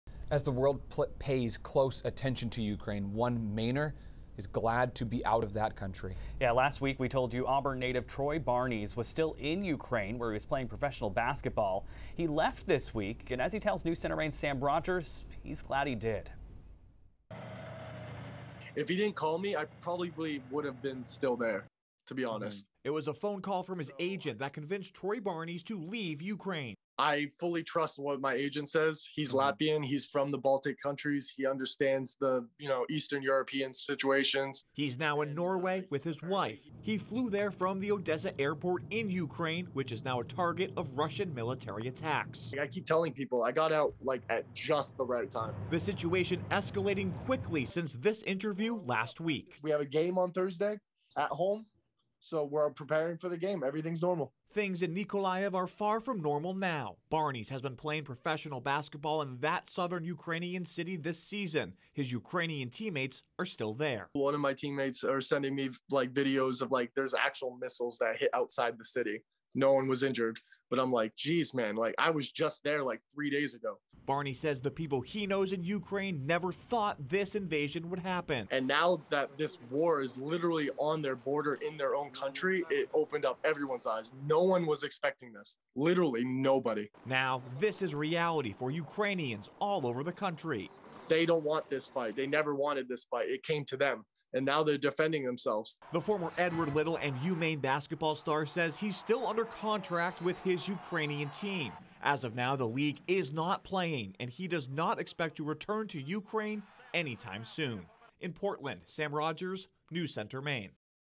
UPDATE: Maine native gets to safety before Ukraine invasion
Troy Barnies has been playing basketball in Ukraine up until he had to evacuate on Monday. He is staying with his wife in Norway.